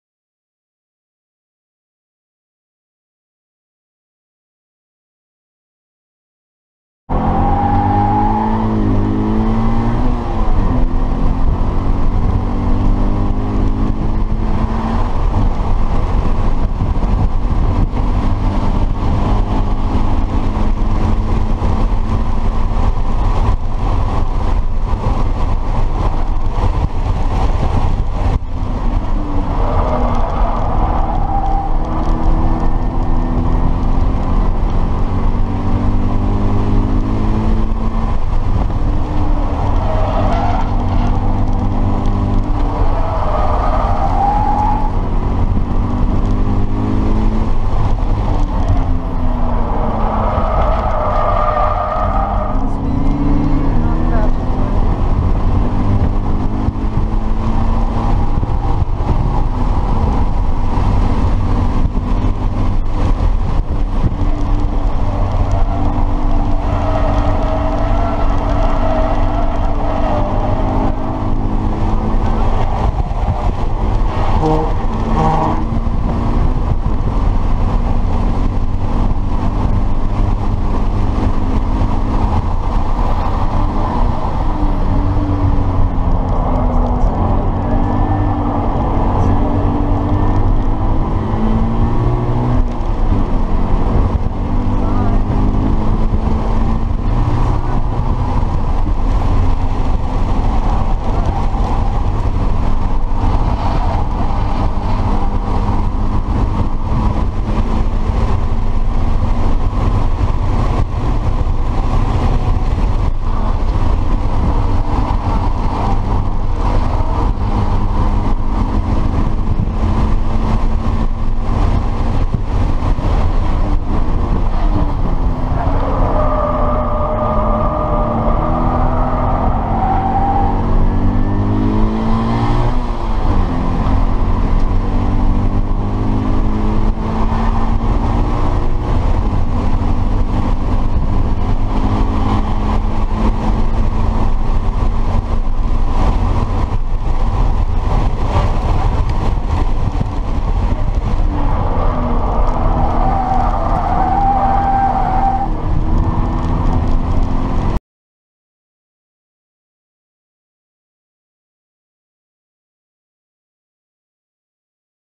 20190525 Sonoma Stock BRZ
2019 Rd 5 86Drive Challenge: Stock Class
New PB 2:02.510
Stock 2013 BRZ w/camber bolts, better brake pads/fluid and oil cooler.
GT Radial SX2 225ZR17 (TW260)